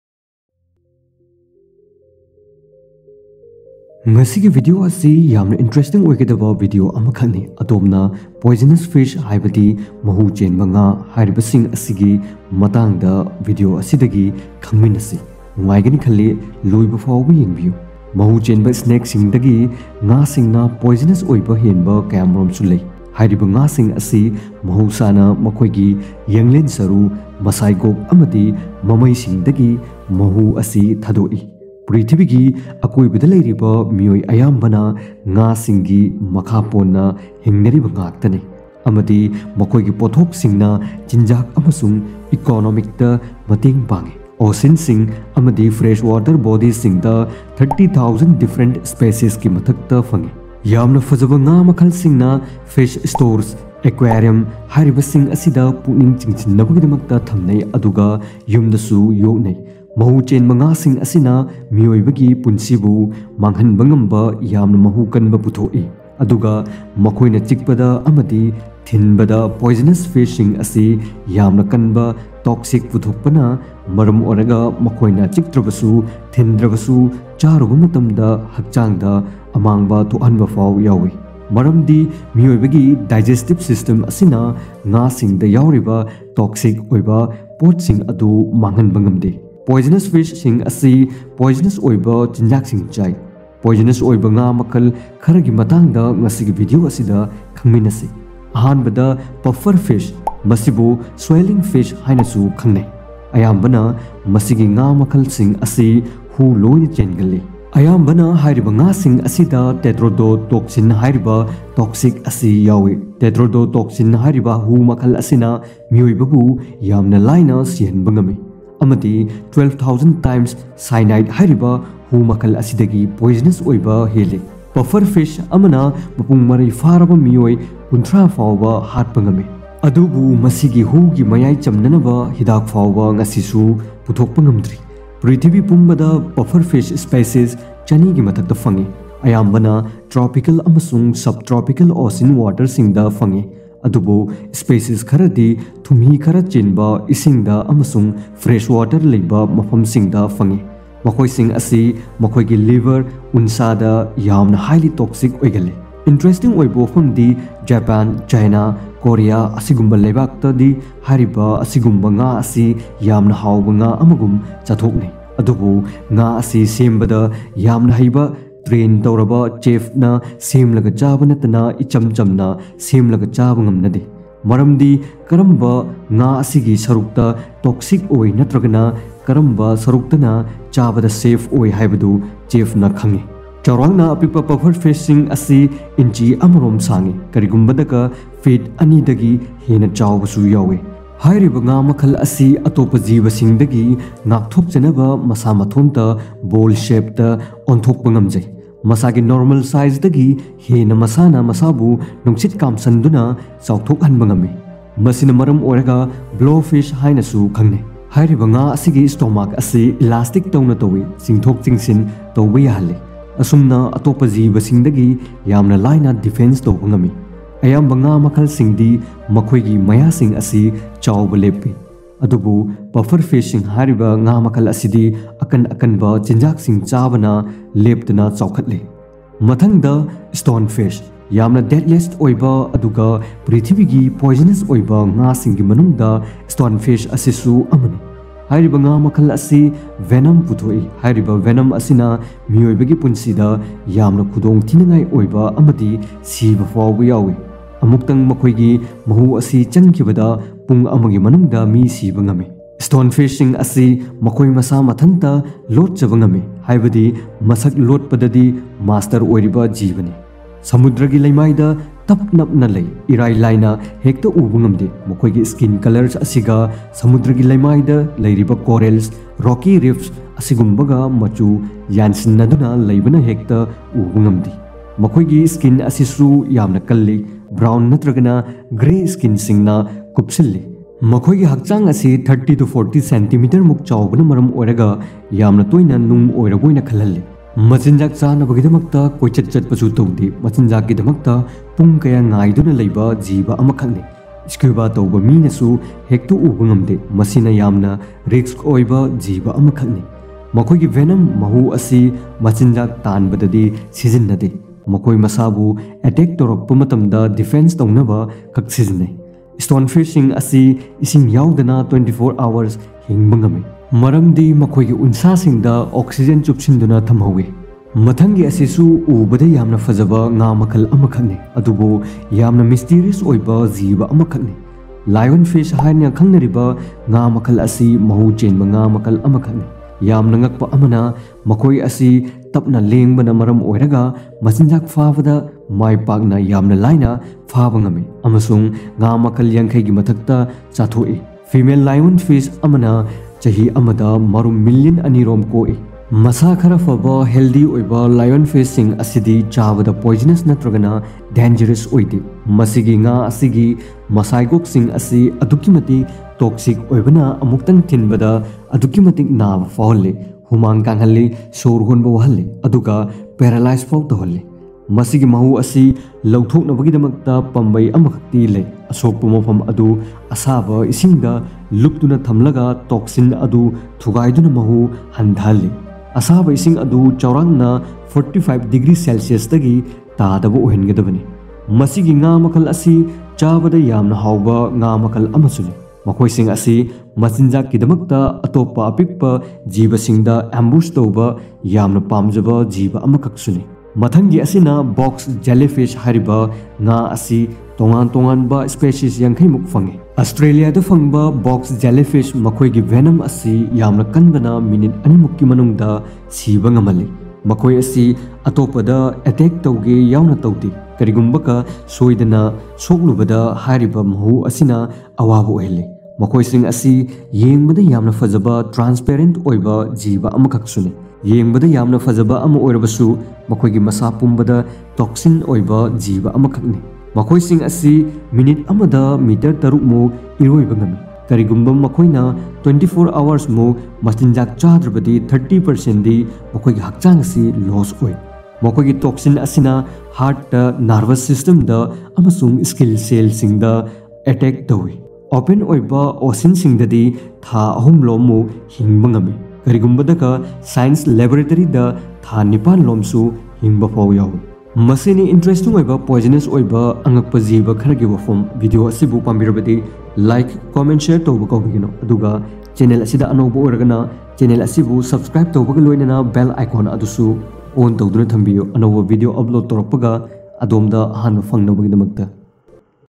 Khuwaidagi Hu Chenba Nga-sing || Most Poisonous Fish In The World || Facts
This video is based on Internet Research

Motivate Oinaba Thanks

Etao Gi Gyaan

From : Manipur (India)